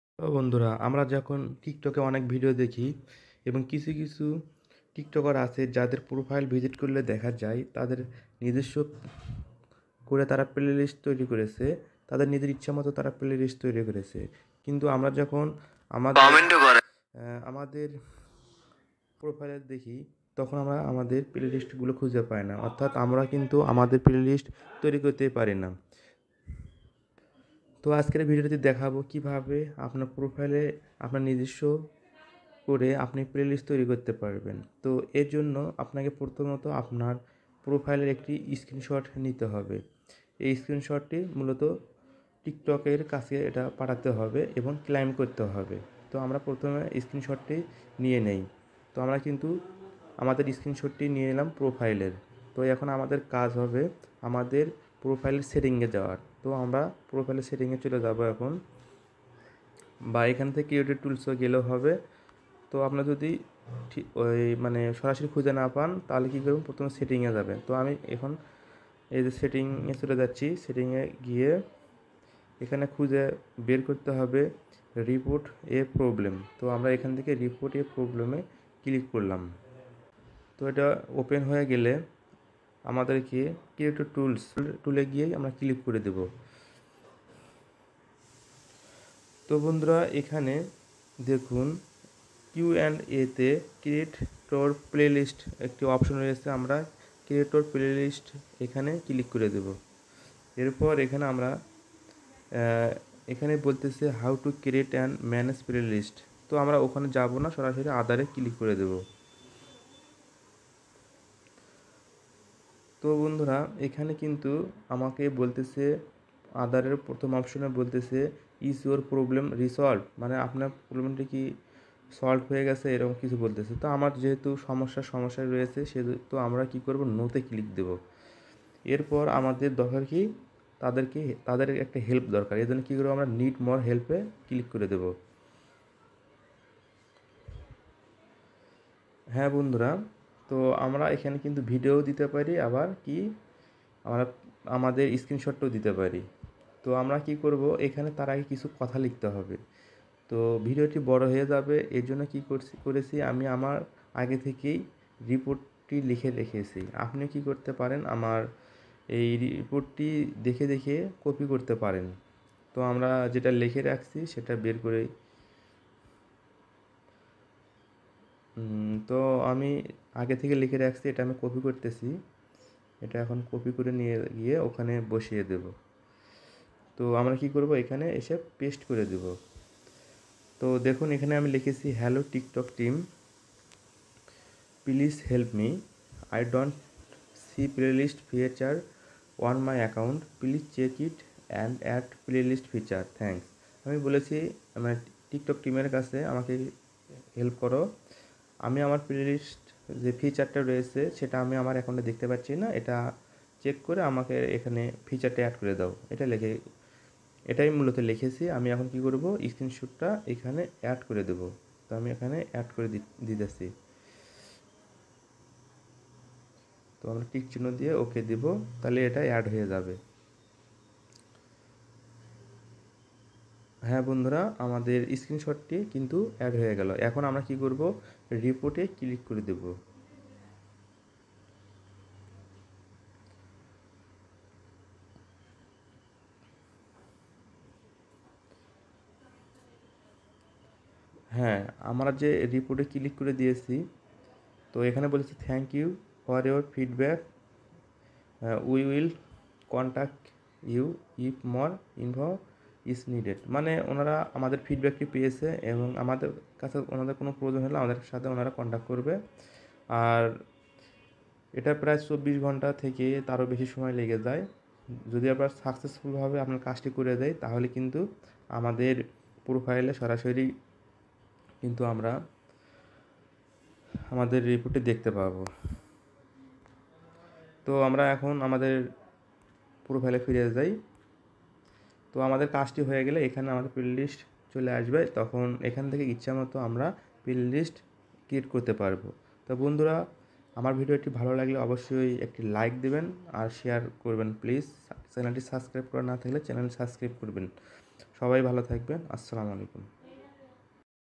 How to create Tiktok playlists in Bangla | কীভাবে নিজের টিকটক প্লেলিস্ট তৈরি করা করব | 10 Ana Blog
Tiktok playlists feature খুবই গুরুত্বপূর্ণ একজন টিকটক আইডির জন্য। টিকটক প্লেলিস্ট আইডিতে যোগ করা থাকলে দর্শক আপনার ভিডিও গুলো বাছাই করে ইচ্ছেমত দেখতে পারে। Tiktok playlist ফিচারটি থাকলে আপনার ভিডিও গুলো সাজানো গুছানো থাকবে৷ ফলে একজন ভিজিটর যখন আপনার প্রোফাইল ভিজিট করবে, তখন সাজানো গোছানো দেখে ফলো করে যাবে।
তাই আপনার উচিত খুব শীঘ্রই টিকটক প্লেলিস্ট যোগ করা।